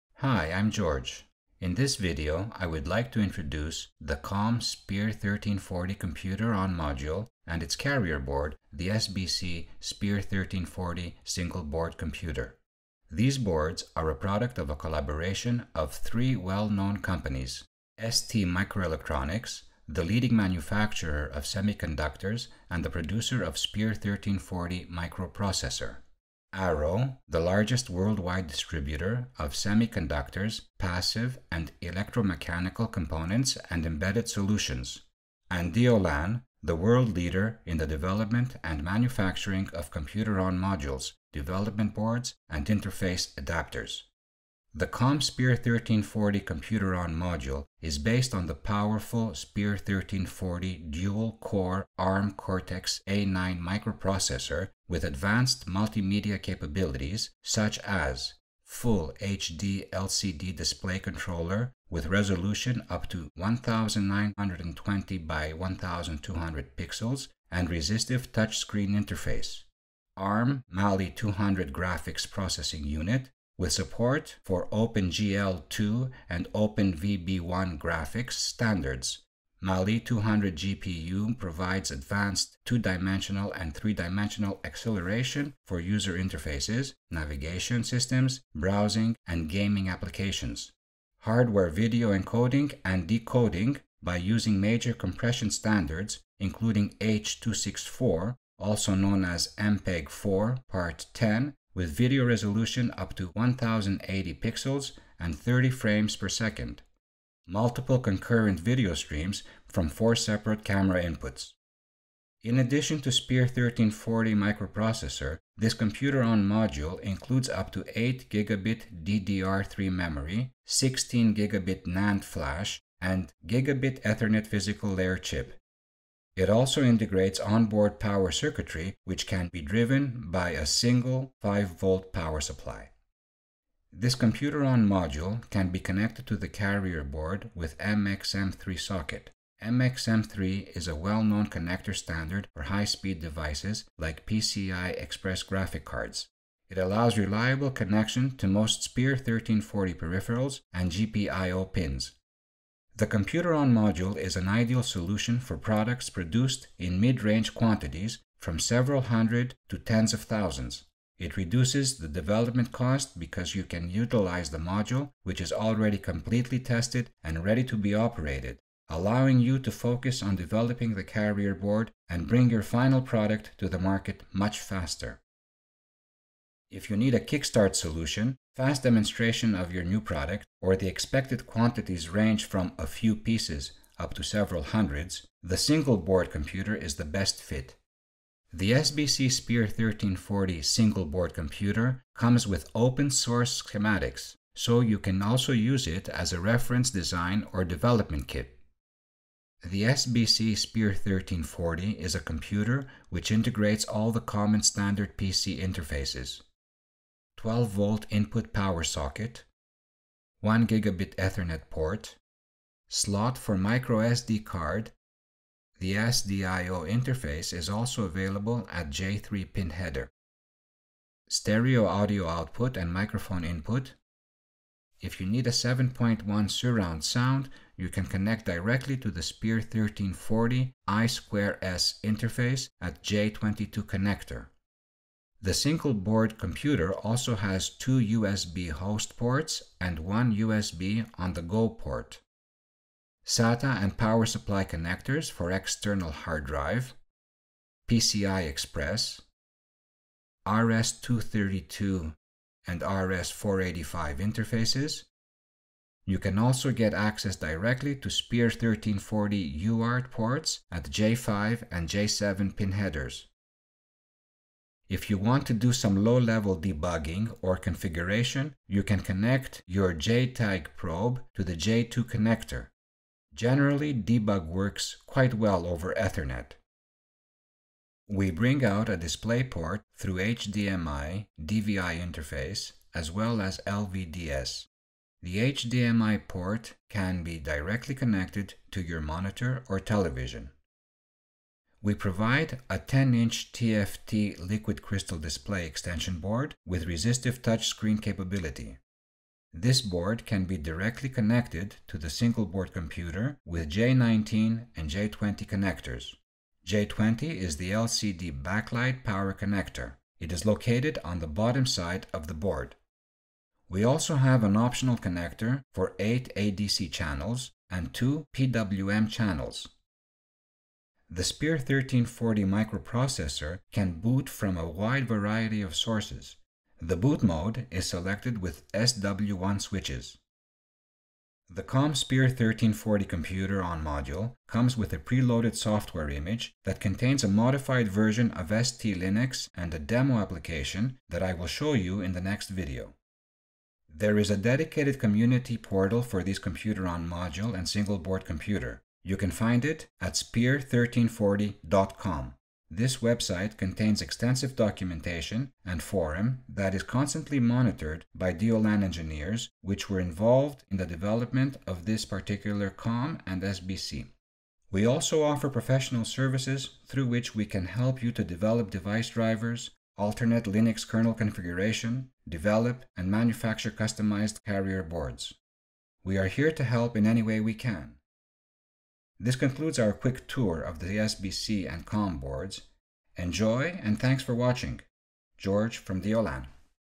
Computer on Module COM-SPEAR1340
An introduction of the COM-SPEAR1340 computer on module and SBC-SPEAR1340 single board computer.
- the community portal for this computer on module.